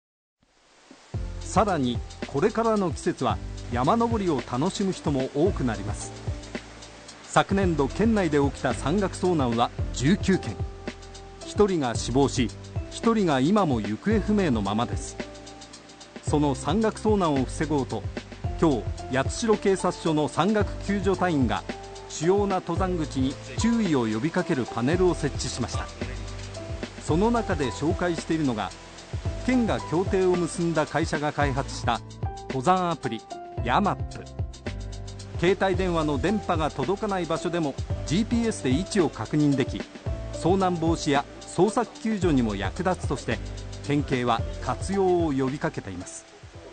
ＧＷ山岳遭難に注意！！「アプリ活用を」 2022年度、熊本県内の山岳遭難は19件（1人が死亡・1人がいまも行方不明のまま）
これからの季節は、山登りを楽しむ人も多くなります。2022年度、熊本県内で起きた山岳遭難は19件。1人が死亡し、1人がいまも行方不明のままです。その山岳遭難を防ごうと4月28日、八代警察署の山岳救助隊員が、主要な登山口に注意を呼び掛けるパネルを設置しました。その中で紹介しているのが、熊本県が協定を結んだ会社が開発した登山アプリ「YAMAP（ヤマップ）」。携帯電話の電波が届かない場所でもGPSで位置を確認でき、遭難防止や捜索救助にも役立つとして、熊本県警は活用を呼び掛けています。